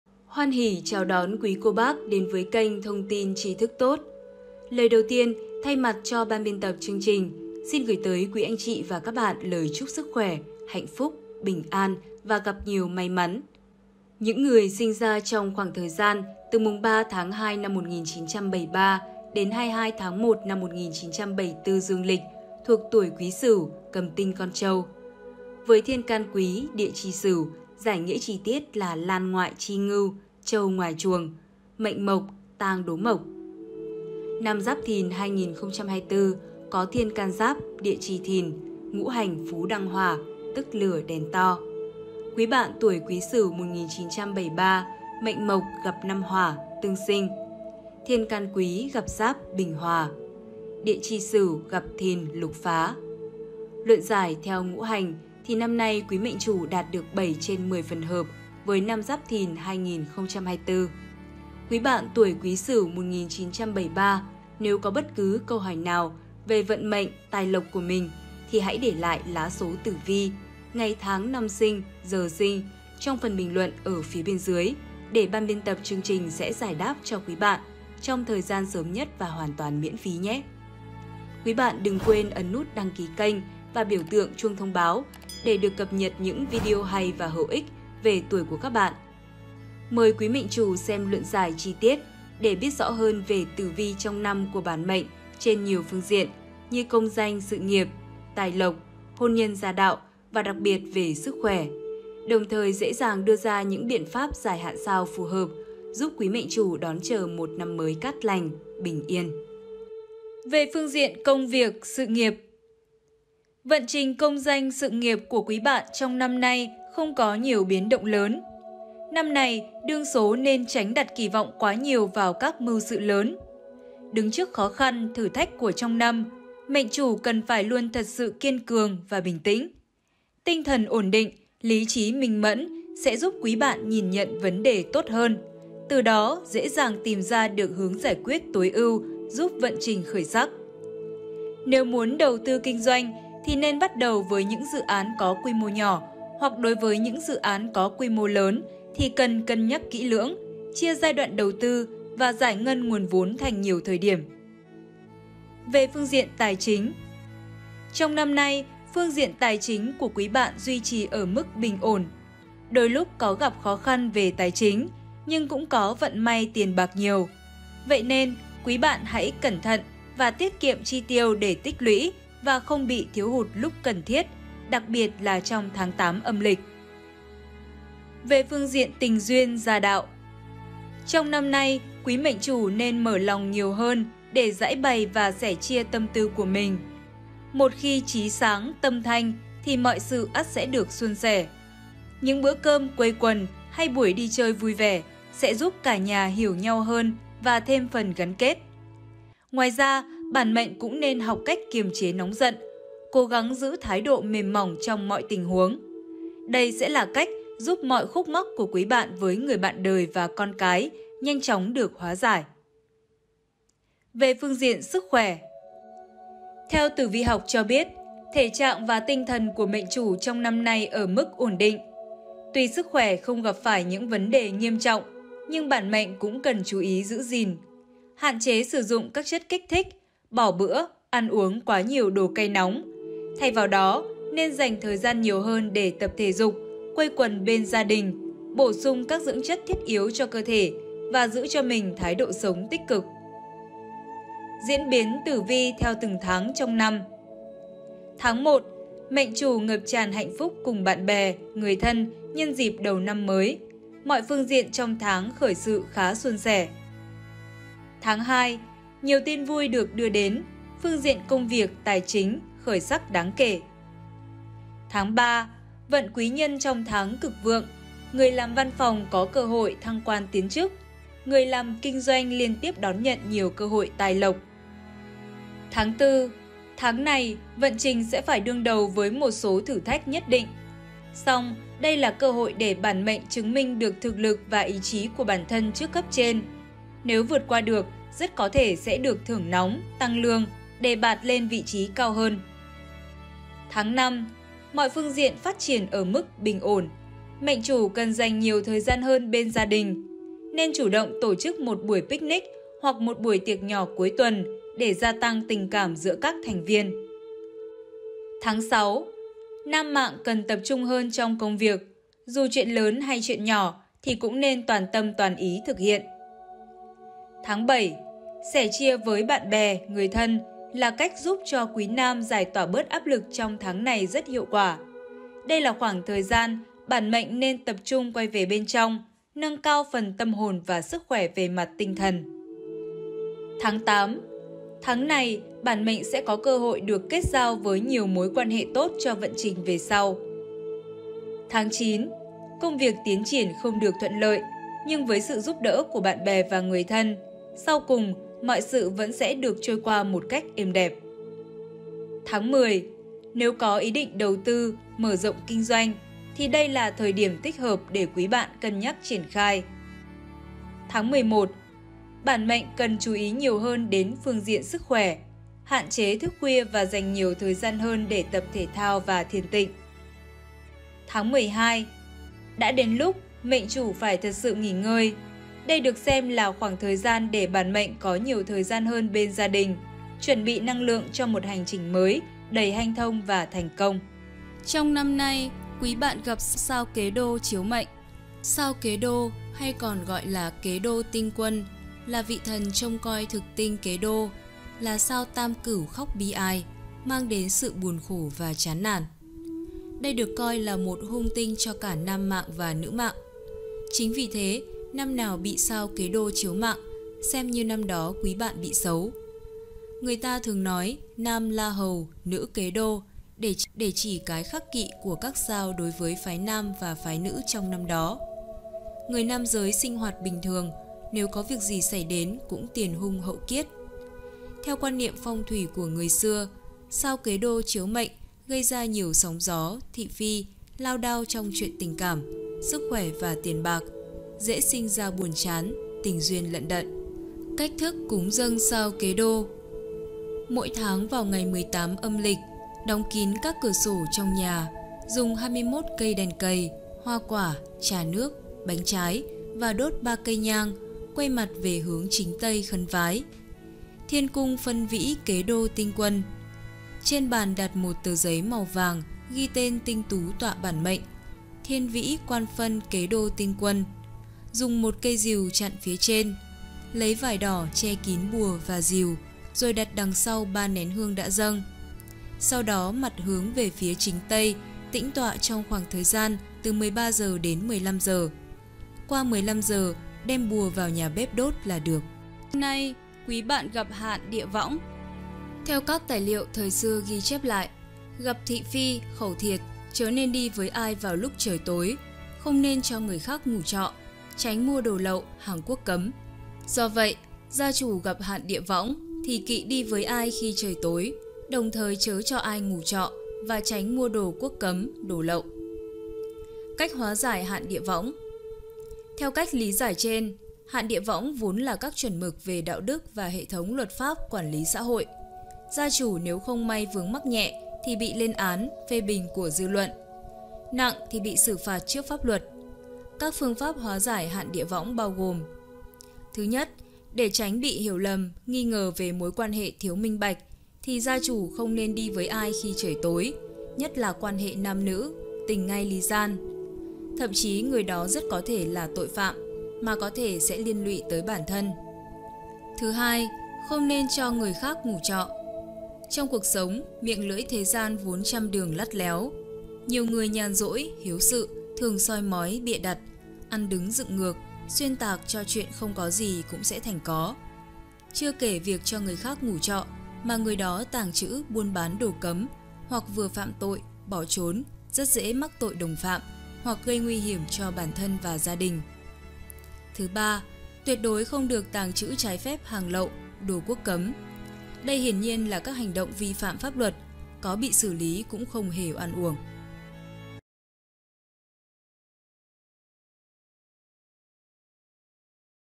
Tử Vi Tuổi Quý Sửu 1973 Nam Mạng Năm 2024 - Vượt Sóng Gió
🔥 Nội dung bài chia sẻ về Tử vi của tuổi Quý Sửu 1973 Nam Mạng trong năm 2024 bao gồm 3 phần:
👉🏻 Phần 1 - Luận giải tử vi tuổi Quý Sửu 1973 Nam Mạng trong năm 2024 trên các phương diện: Vận trình Công danh sự nghiệp, Vận trình tài lộc, Vận trình tình duyên gia đạo, Vận trình sức khỏe.
👉🏻 Phần 2 - Luận giải Tử Vi tuổi Quý Sửu 1973 Nam Mạng theo từng tháng trong năm 2024.
👉🏻 Phần 3 - Cách cúng dâng sao và hóa giải vận hạn cho tuổi Quý Sửu 1973 Nam Mạng trong năm 2024.

Kênh YouTube: Vượng Tài Lộc là kênh chia sẻ về tử vi, phong thủy, xem tướng số, phật giáo tâm linh.

© Bản quyền thuộc về Vượng Tài Lộc
© Copyright by Vượng Tài Lộc Production ☞ Do not Reup